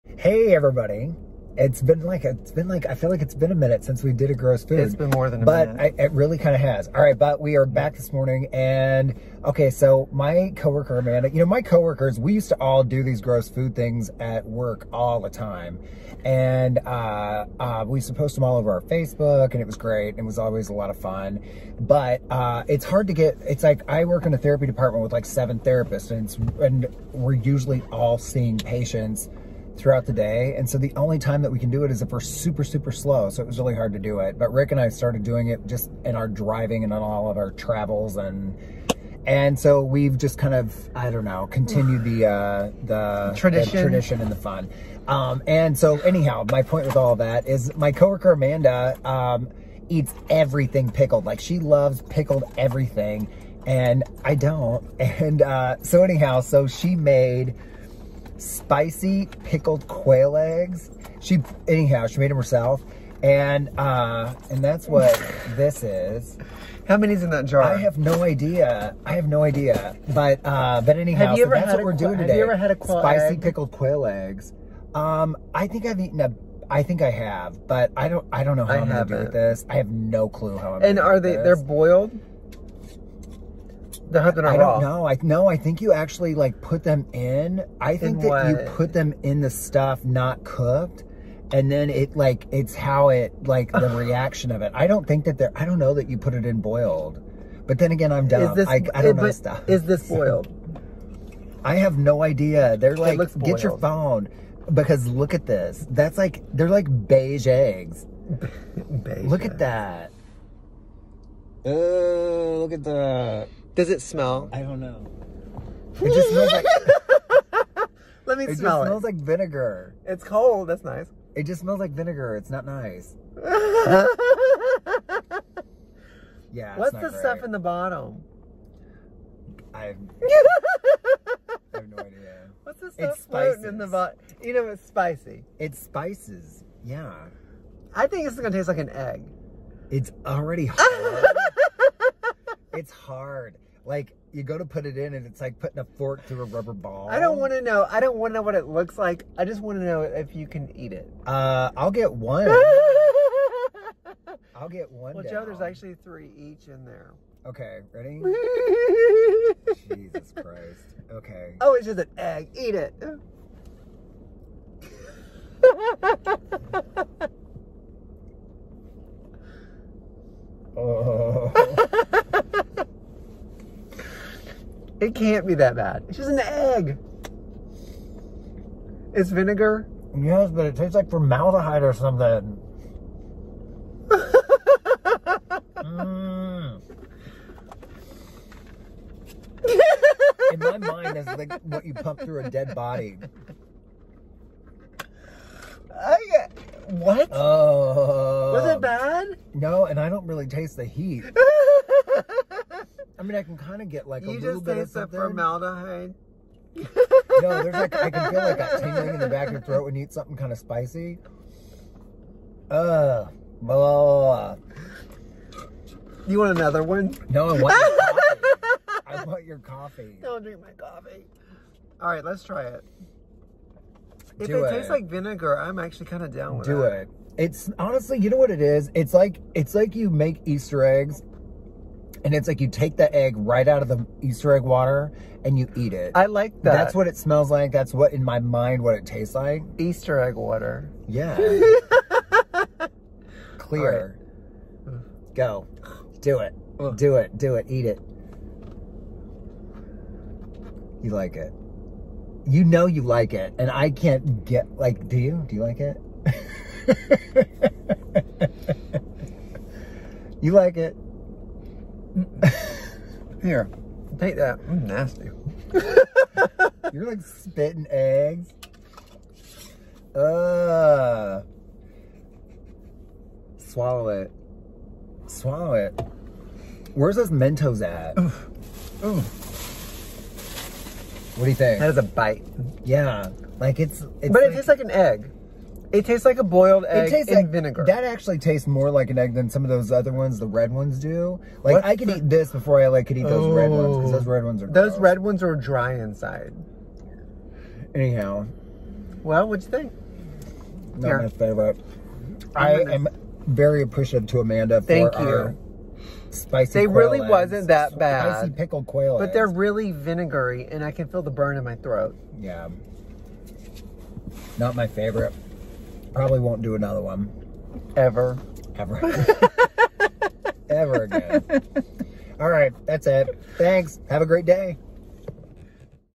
Joe & Rick Food Challenge!!🤣🤢🥴 Spicy Pickled Quail Eggs 🌶🥚🌶
We totally forgot to post this one.  A BIG THANK YOU to Amanda for making us a batch of spicy pickled quail eggs.  Is it just southerners who eat stuff like this?  Are people somewhere making Blue Jay frittatas and we’re missing out??